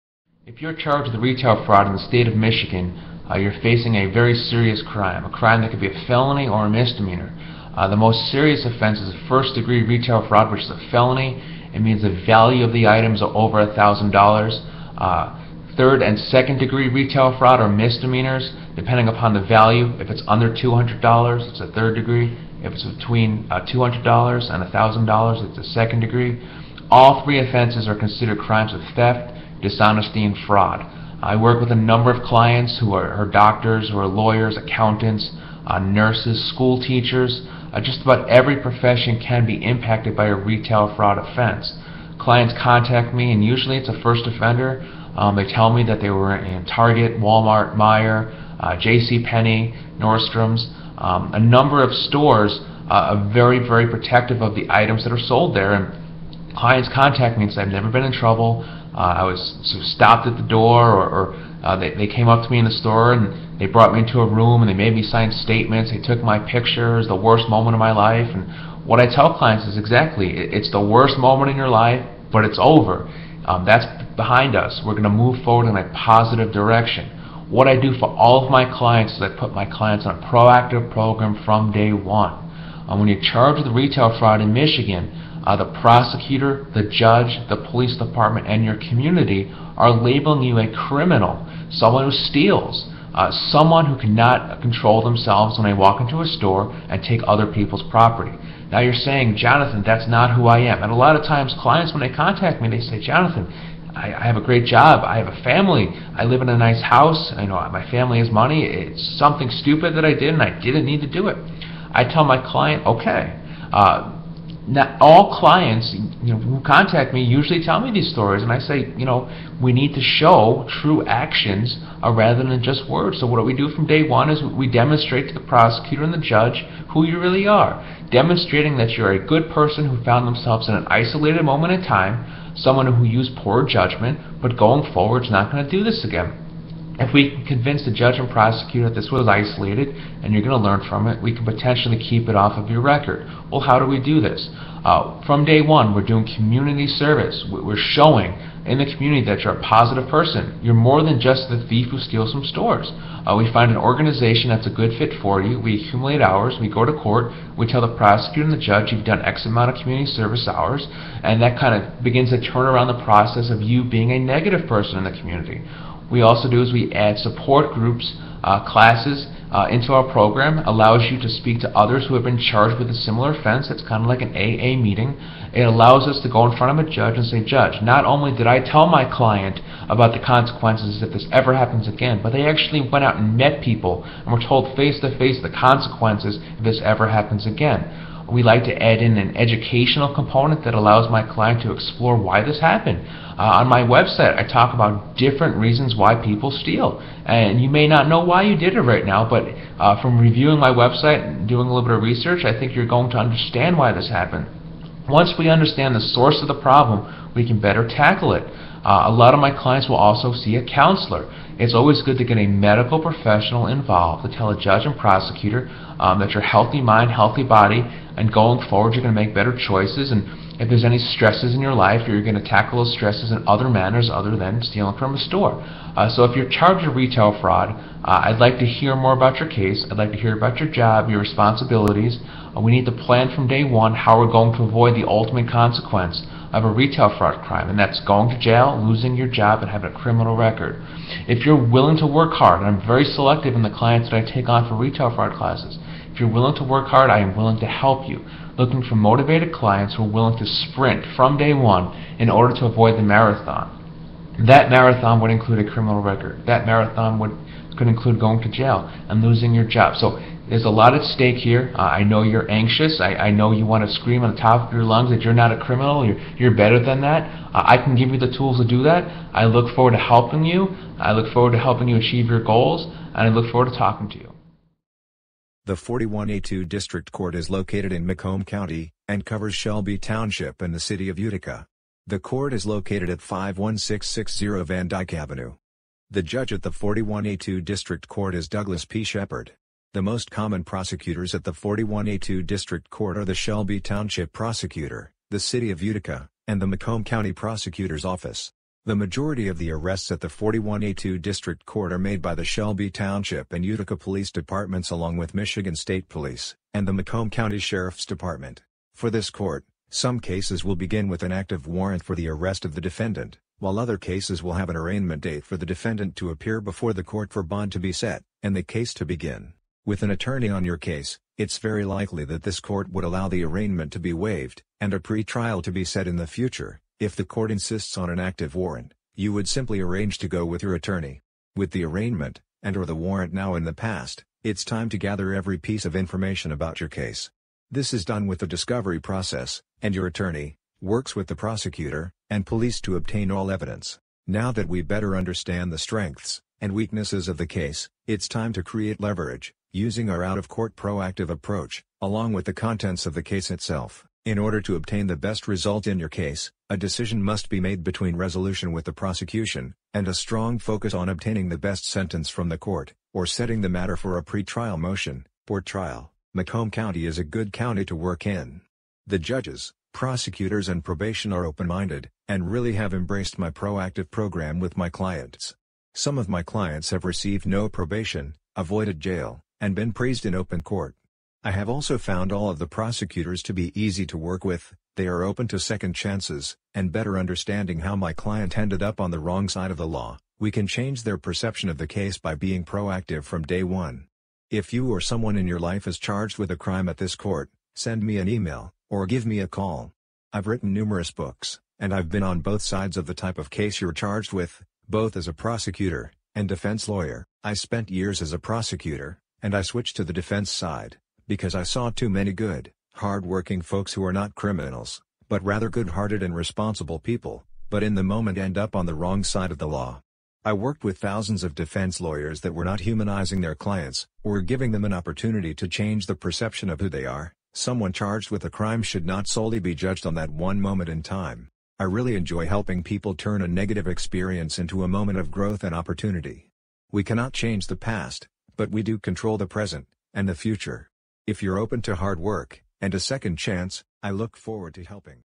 Retail Fraud Shelby Township - Shoplifting Macomb County Judge Shepherd
If you're charged with retail fraud in Michigan, the prosecutor doesn't trust you.  The allegation is you stole something from a store, and the prosecutor will assume you were finally caught after years of stealing from multiple stores.

This may or may not be true, but it really doesn't matter, because there is no way to show this isn't the truth; perception runs wild, because the prosecutor can't imagine stealing in their own life as they've kept a straight arrow lifestyle to go on to prosecute cases for a living.  This is a crime of fraud, theft and dishonesty and it puts you in a negative light to the government.

Prosecutors also assume these cases are pretty cut and dry; the police report says that loss prevention officer #1 observed the defendant doing X, Y and Z, and hey there's even a video of the incident.  Maybe the defendant even admitted to stealing when they were caught.  Wow this is an easy case, why wouldn't the defendant just run to the courthouse and plead guilty.

A young prosecutor may not be thinking outside the box and realize that loss prevention officers go on vacation, get fired, relocate, quit their jobs.  There may be strong evidence of the crime, but if the witness isn't around, then they have no case.

As a defense attorney, we acknowledge the perception of our audience; we acknowledge that the prosecutor believes they have a strong case and there may be no room for negotiations on the legal issue.  We do our due diligence with the evidence, but we focus on creating leverage outside of the courtroom by being proactive.  It's likely that the client is very guilty, and well that's OK.  I don't need an innocent client as a defense lawyer to win the case.

We need to show that this can be a valuable learning experience for my client; we need to figure out why they stole.  We can't throw our hands up in the air and say "we don't know" because that's unpredictable and dangerous to toss my client back into society now knowing why they break the law, because nothing has been done to stop it from happening again.  We identify the issue, and work on it.  We don't run from it, we embrace the opportunity to be a better person going forward.

We can't go back and change the past, but we control the present, and can project the future for the prosecutor and the judge.  We tailor 3-4 proactive steps for the client that allow them to show the other side of them; it is not fair to be judged based upon a single police report.  It's only one small factor that goes into understanding my client.

We work with clients from all backgrounds that are charged with retail fraud cases.  We tend to get a lot of nurses, educators, neglected spouses and other hard working people who at times feel underappreciated for all of their hard work.  Some of these professionals aren't paid what they deserve, and part of that mental state plays a role in how the client finds themselves charged with shoplifting.

Many of our clients are having issues with family, health or working under a lot of stress at the workplace.  Along with the aforementioned clients, we've worked with retail fraud clients who work for the IRS as tax agents and work as medical doctors who steal baseball cards from Target.  Nothing shocks us anymore when it comes to new clients charged with shoplifting.

If the prosecutor and judge get to know my client while working the proactive program, they see the total person, and are more likely to go along with our course of action for the case.  A difficult charge for sure, but a very manageable outcome is likely if the client is ready to work.